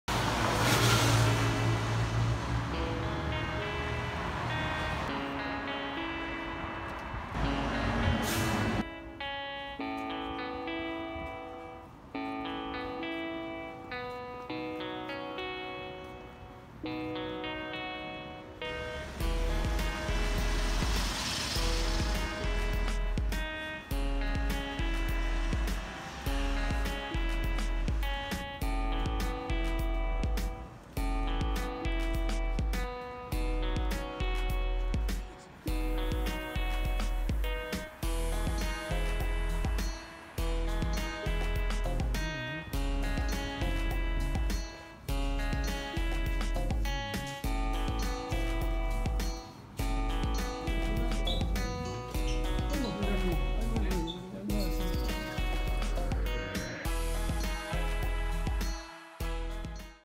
독서모임) 마포나비소풍 망원역에서 가는 방법
마포나비 소풍 가는 방법을 설명해 두었습니다.